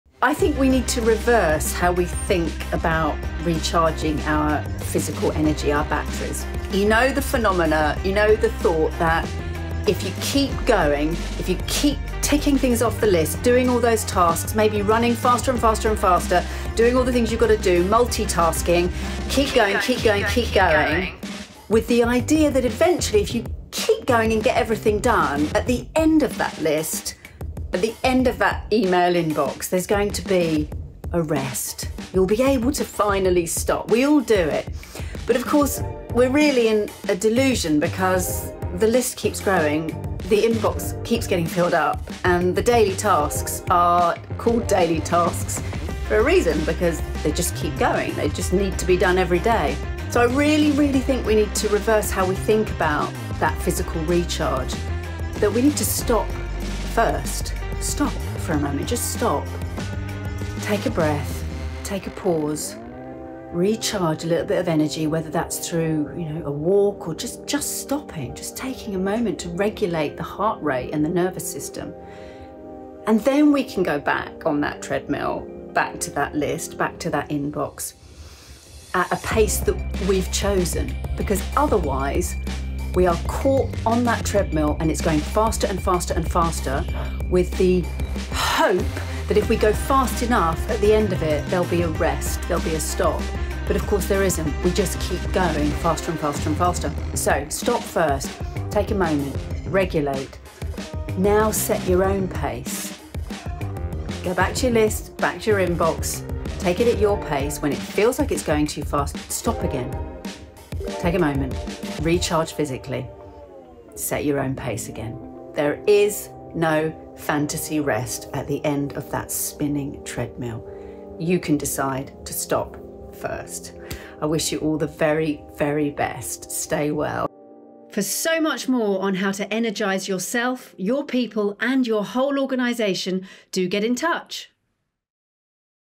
Stop your spinning Treadmill, Start your 4D intentional Living: Reclaiming Your Physical Energy
Feeling caught in an endless loop of tasks, emails, and daily routines? Is it time to take a fresh perspective on managing physical energy? Is it time to challenge our all-too-common belief that rest comes only after completing an ever-growing to-do list?

Its time to escape the ever moving Treadmill...

Constantly chasing the end of your inbox or task list can be a deceptive cycle, and how stopping first can be the key to effective energy recharge. Embrace intentional pauses for heart rate and nervous system regulation, and how these can empower you to tackle your responsibilities at a pace that suits you.

The 'Fantasy reward rest' after the endless, spinning treadmill is just a myth, so it's time to approach your rest from a new angle. Embrace the power of setting your own pace.

Are you seeking practical tips to manage your energy better, reduce stress, and enhance overall wellbeing?